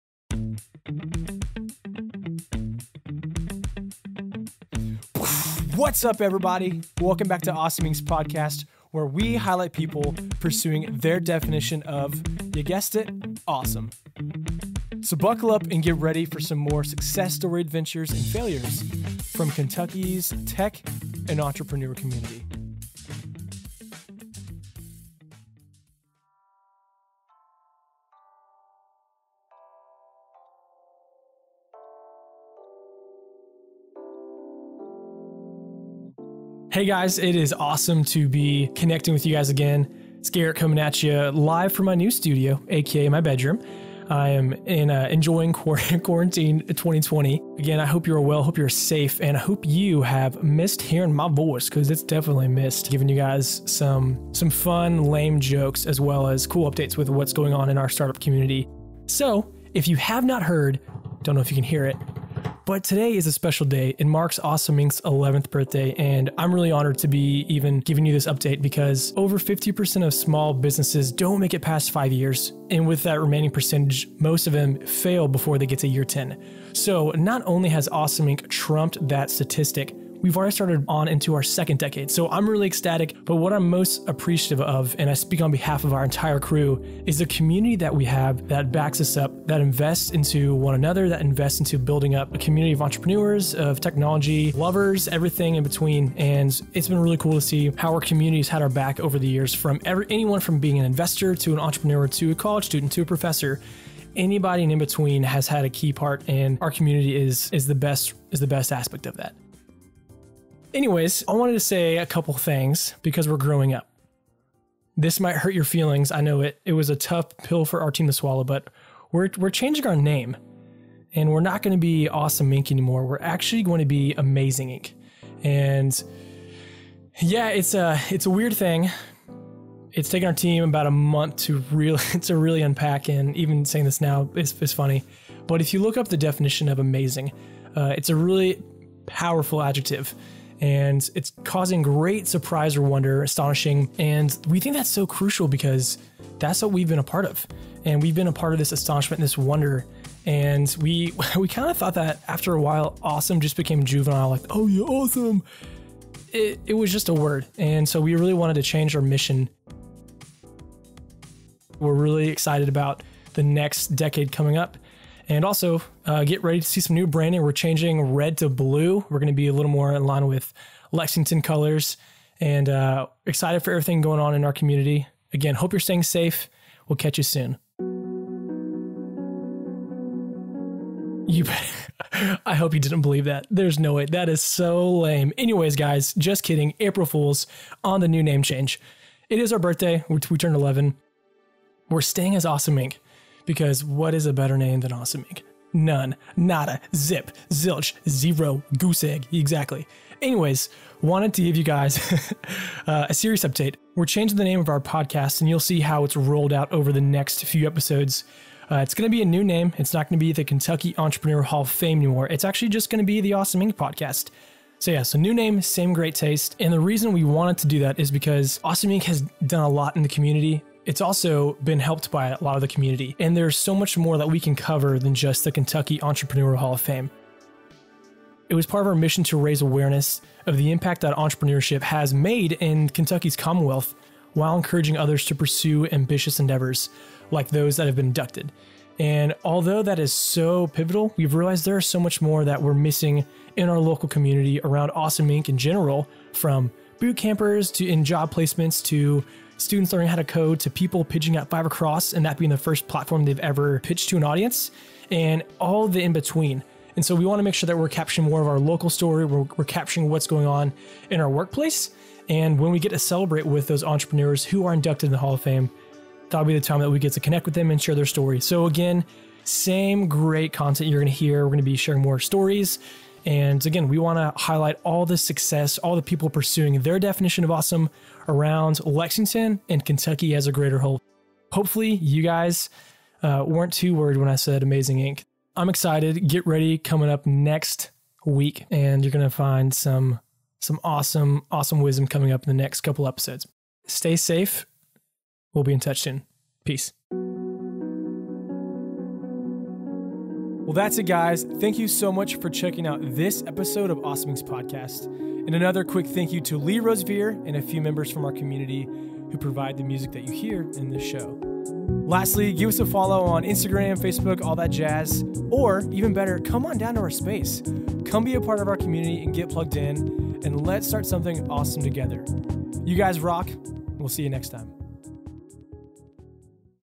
WE'RE CHANGING OUR (PODCAST) NAME | Awesome Inc Podcast Episode 25.5
Some things are changing in house and we want you to stay In the know!

Long story short, we've got some fun and new stuff in the works. We're excited to share more of what our awesome team, startups, and community are doing to make Kentucky the best place to live and work.